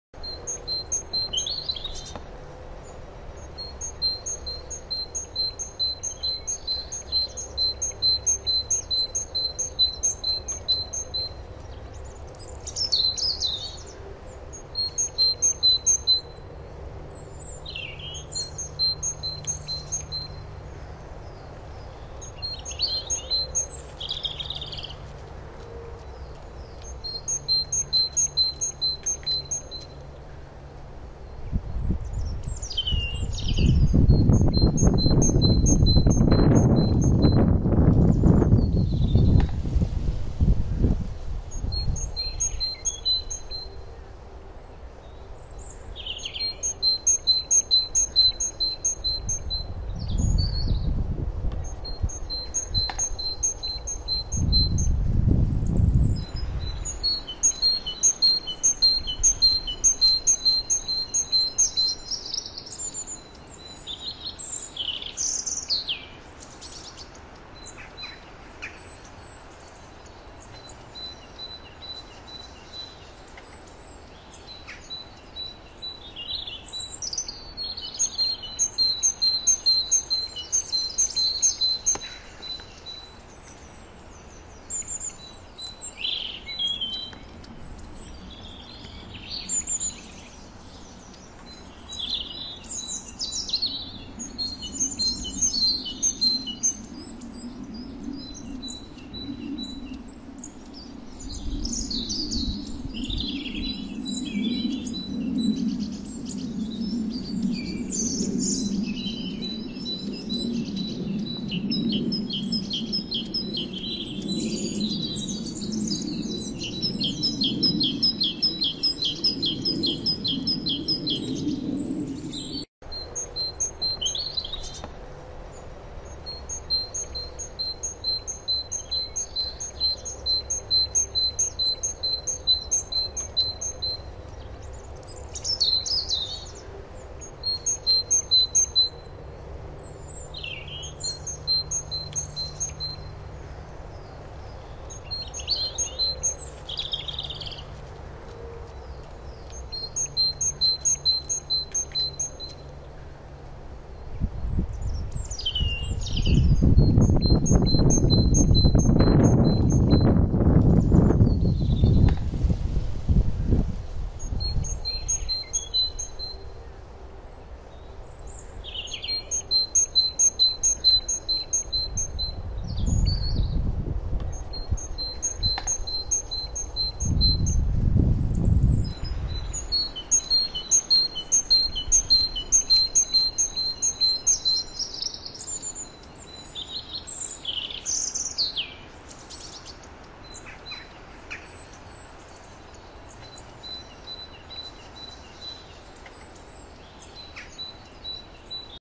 Forest birds at Bob's feeder 19-11-2021
Forest birds at Bob's feeder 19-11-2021 intaking energy supplies before winter.